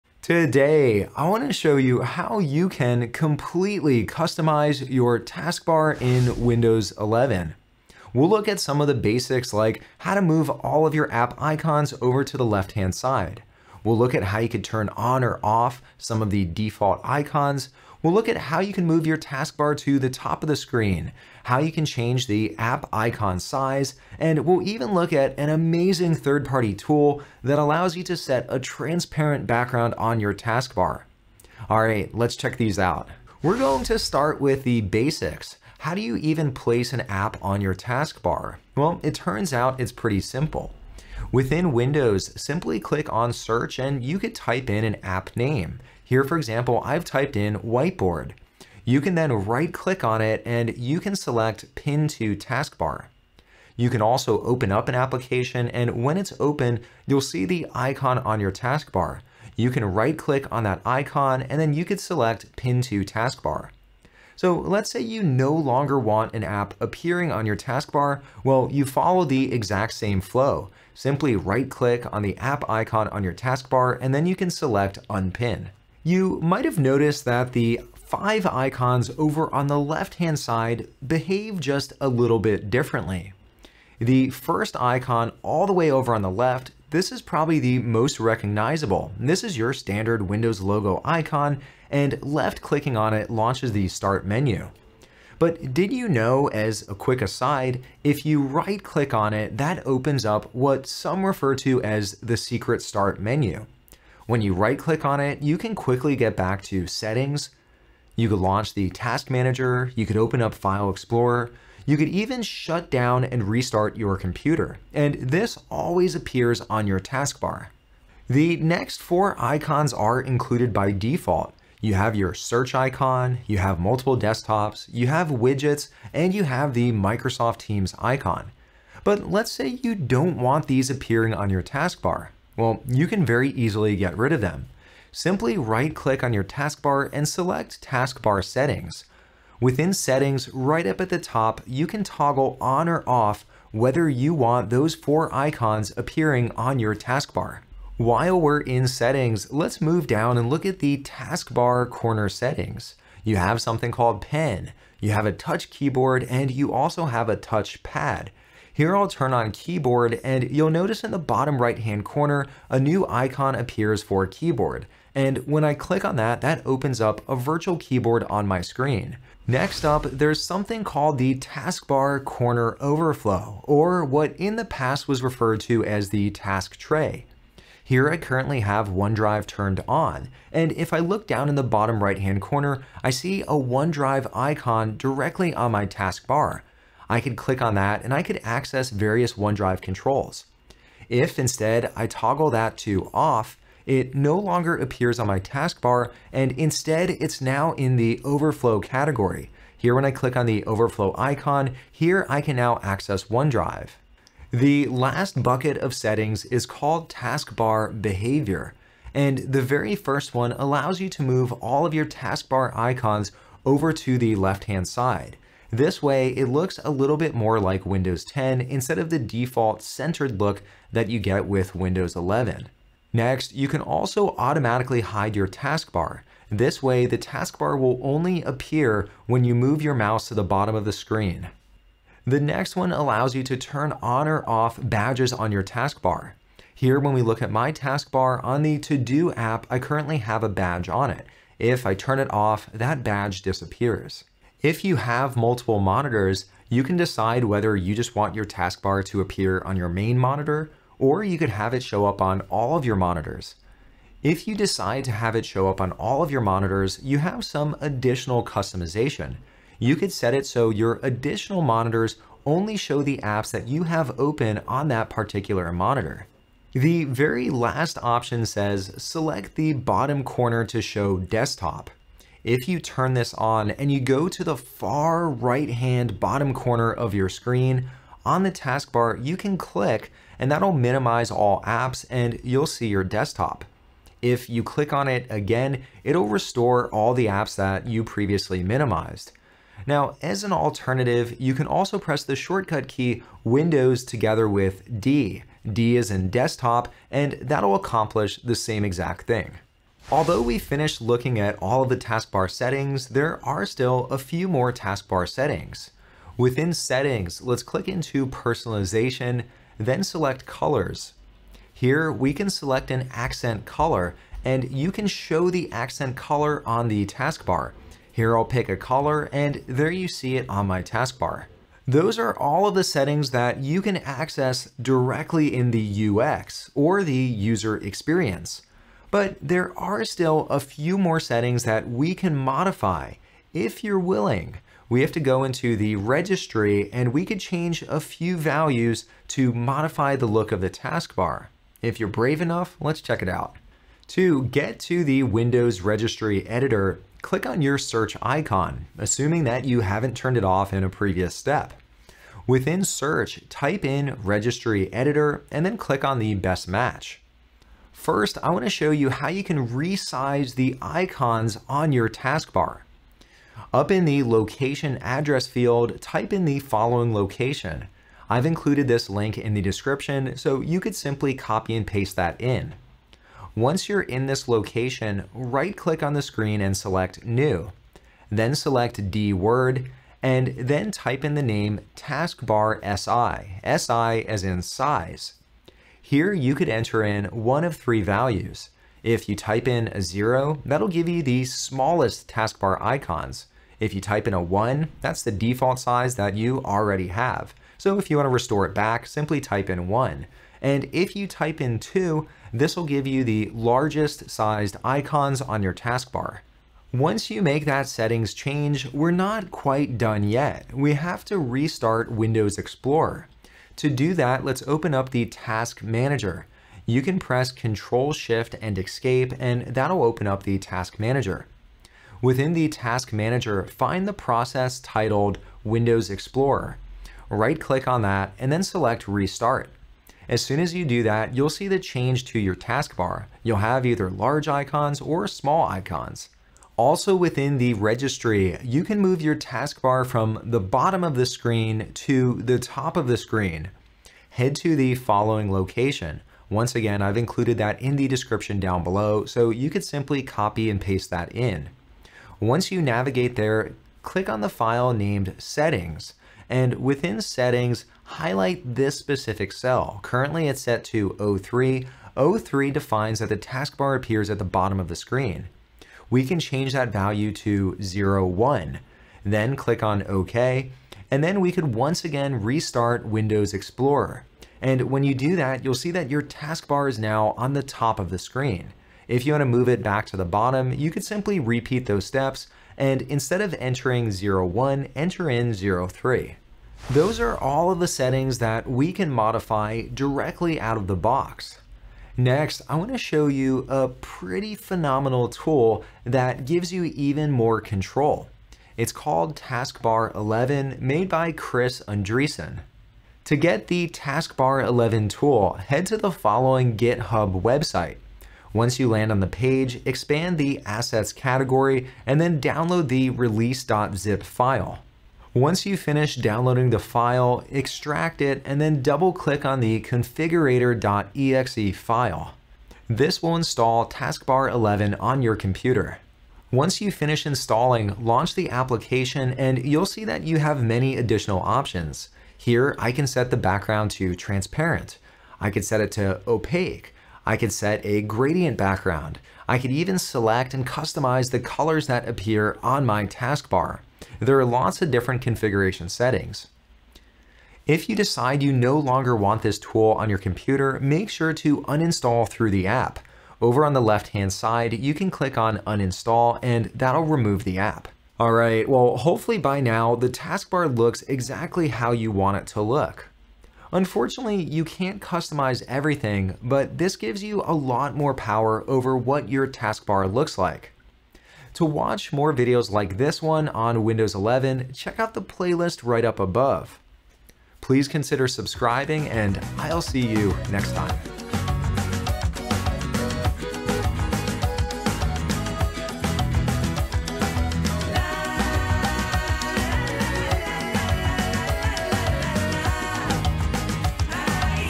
Windows 11 Taskbar Customization Tutorial
In this step-by-step tutorial video, learn how you can fully customize your task bar in Windows 11. Learn the basics like how to pin and unpin apps. Find out how to move your task bar icons to the left or back to the center. We also explore how to remove default app icons, like search, multiple desktops, widgets, and Teams. We look at some more advanced task bar tweaks that require modifying the registry. By modifying the registry, we can adjust the size of the task bar icons as well as place the task bar at the top of the screen. Lastly, we look at a phenomenal free tool that allows you to use a transparent background with your task bar. By the end, you'll have full control over all the ways to modify the look and feel and the behavior of your Windows 11 task bar.

👋 Additional resources
- Resize icons on taskbar:
Location in registry: HKEY_CURRENT_USER\Software\Microsoft\Windows\CurrentVersion\Explorer\Advanced
Click New and then DWORD (32-bit) Value
Set the name of the DWORD Value as “TaskbarSi” and hit the Enter key to save changes.
Small taskbar – TaskbarSi value 0
Default taskbar – TaskbarSi value 1
Large taskbar – TaskbarSi value 2

- Move taskbar to top of screen
Location in registry: HKEY_CURRENT_USER\Software\Microsoft\Windows\CurrentVersion\Explorer\StuckRects3
00 - Left (this doesn't currently work)
01 - Top
02 - Right (this doesn't currently work)
03 - Bottom

⌚ Timestamps
0:00 Introduction
0:33 Pin & unpin apps
1:15 Unpin search, multiple desktops, widgets & Teams
2:27 Taskbar corner icons
2:49Taskbar corner overflow
3:23 Left align taskbar icons
3:42 Automatically hide taskbar
3:52 Show badges on taskbar apps
4:04 Taskbars & multiple monitors
4:26 Show desktop icon
5:21 Adjust icon size
7:16 Move taskbar to top
8:06 Set transparent background
9:17 Wrap up